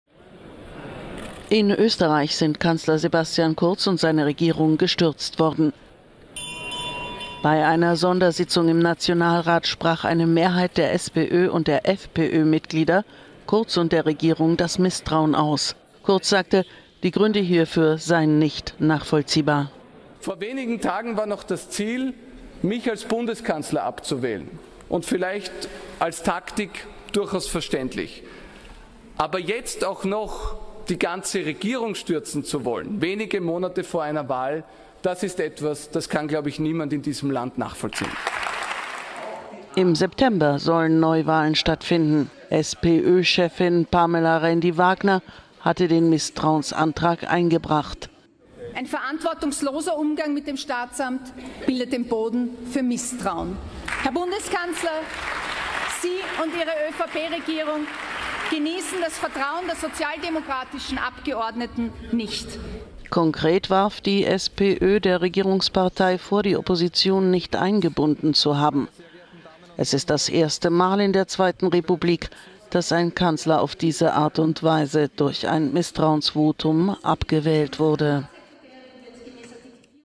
Kanzler Kurz verliert Misstrauensvotum - was folgt jetzt?
In Österreich haben SPÖ und FPÖ Kanzler Sebastian Kurz zu Fall gebracht.…